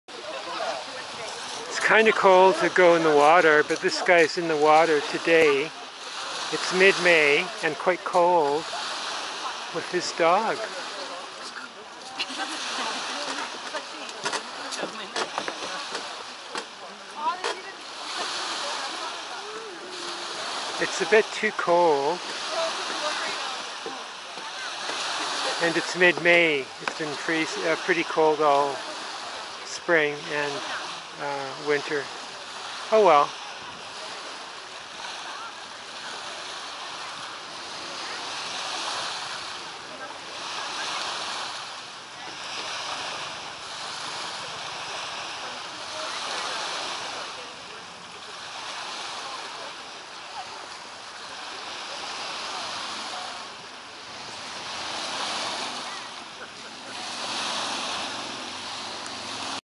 Swimmer and dog enjoy a swim in the ocean in White Rock even though the water is cool in May.
Swimmer and his dog enjoy a swim in uncrowded White Rock